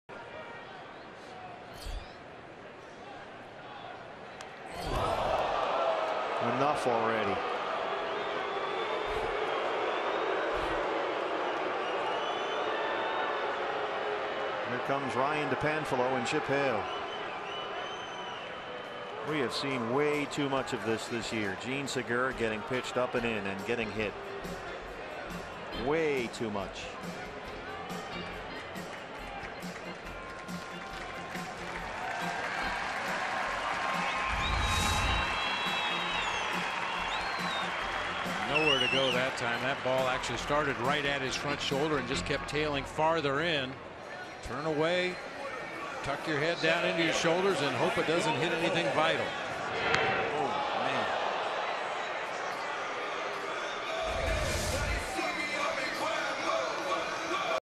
ATL@ARI: Segura stays in the game after hit-by-pitch
Jean Segura gets checked out by trainers after getting hit on the shoulder with a pitch, but remains in the game in the 7th